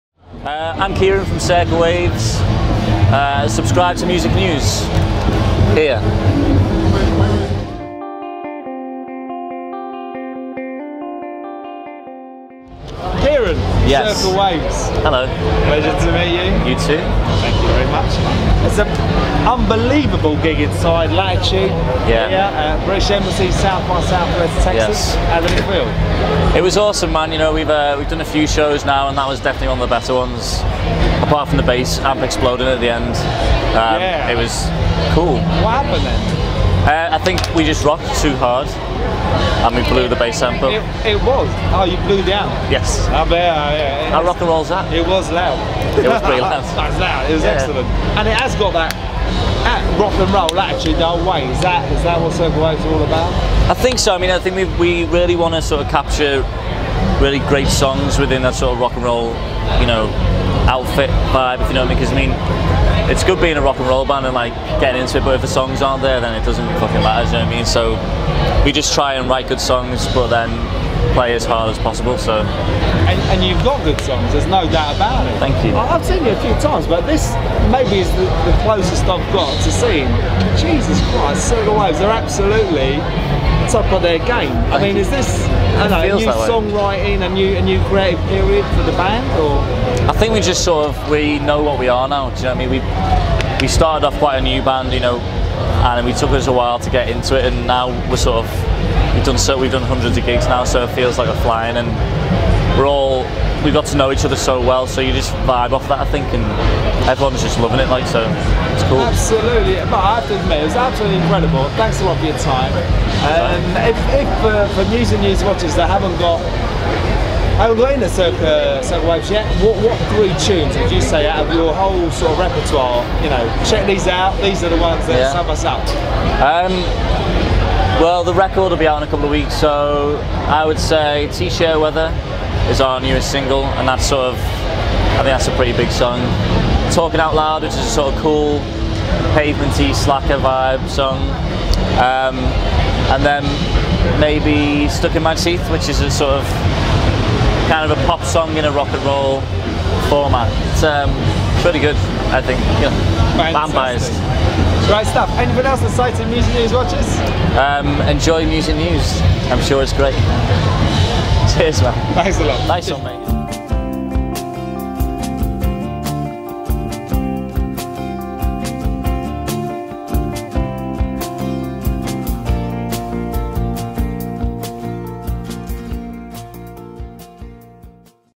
Circa Waves I Interview I Music-News.com
Kieran Shudall of Circa Waves interviewed by Marco Gandolfi editor of Music-News.com at Latitude, SXSW, Austin, Texas on March 19th 2015.

Circa Waves are: Kieran Shudall (guitar / vocals), Joe Falconer (guitar), Sam Rourke (bass) & Colin Jones (drums).